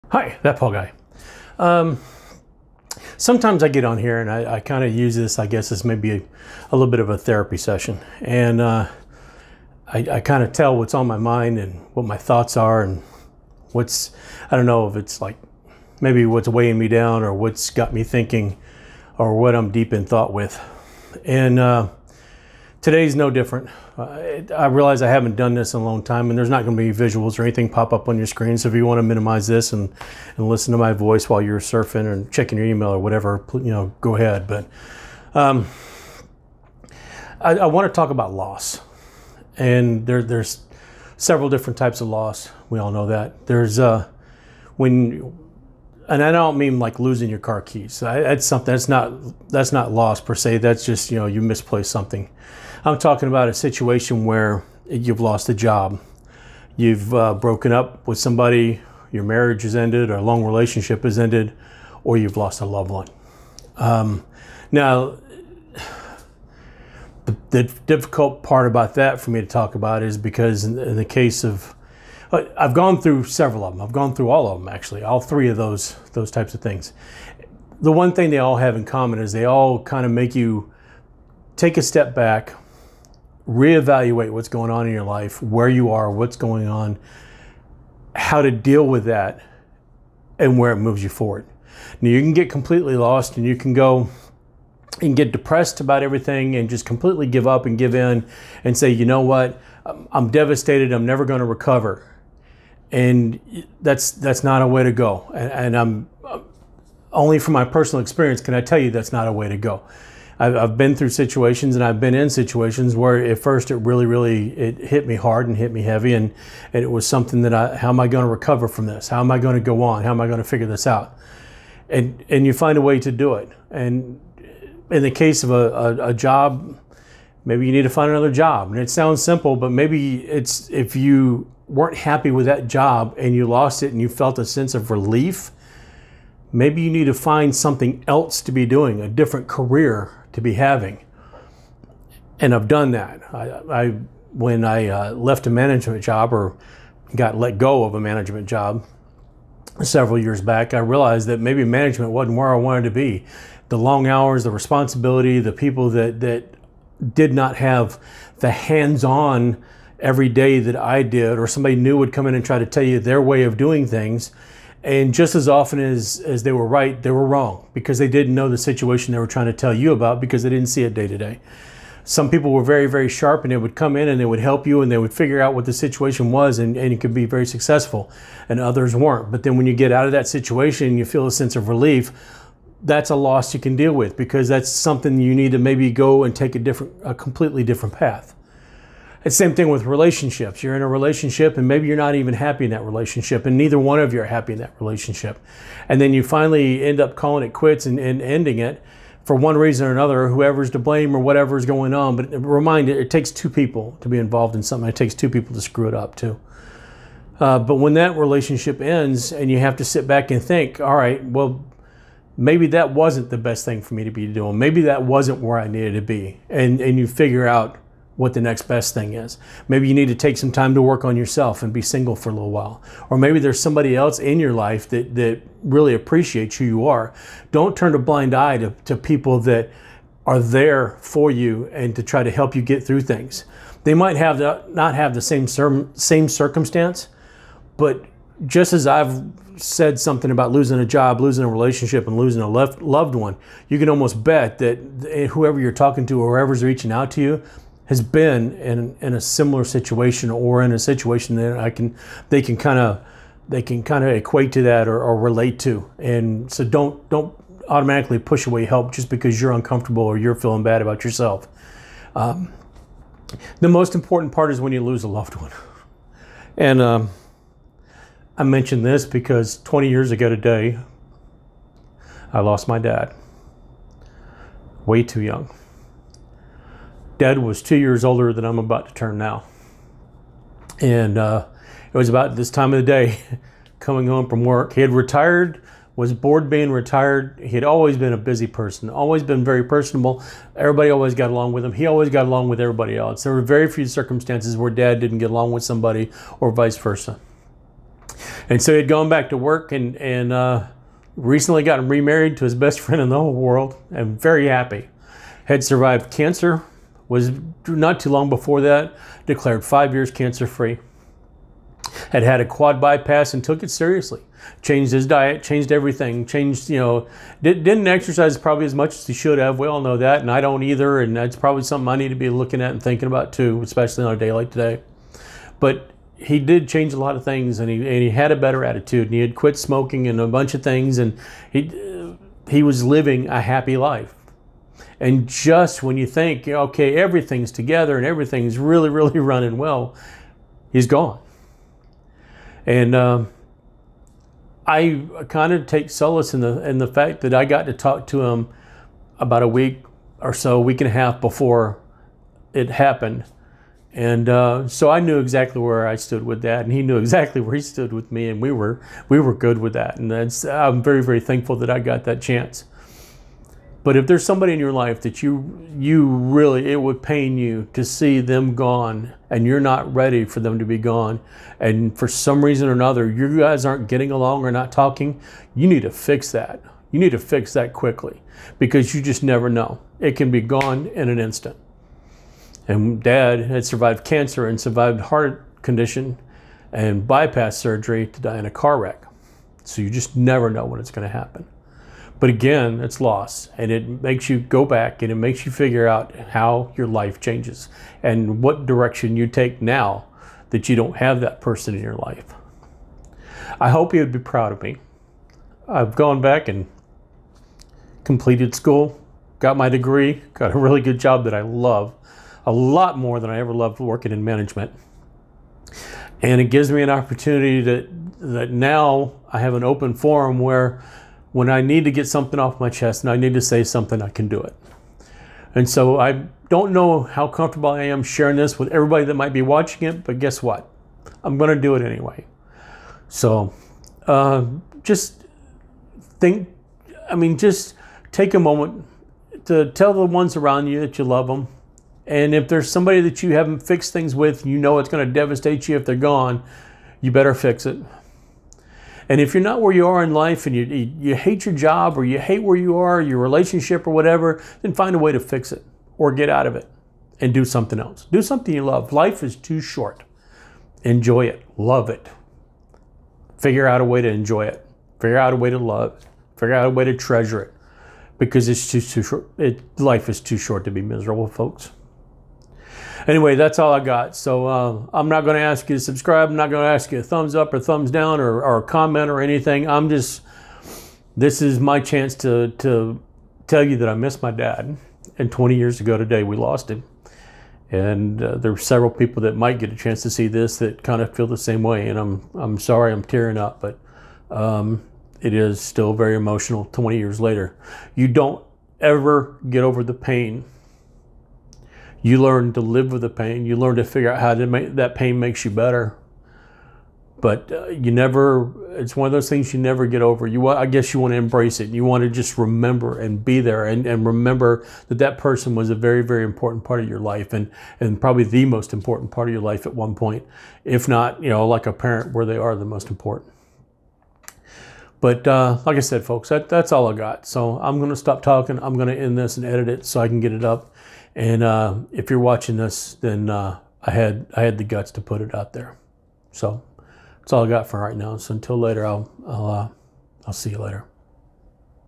Dealing with Loss. What do you do when you face a Major Life Change like the passing of a loved one.
Just my random thoughts and feelings today. I have a very heavy heart because today marks twenty years since Dad passed.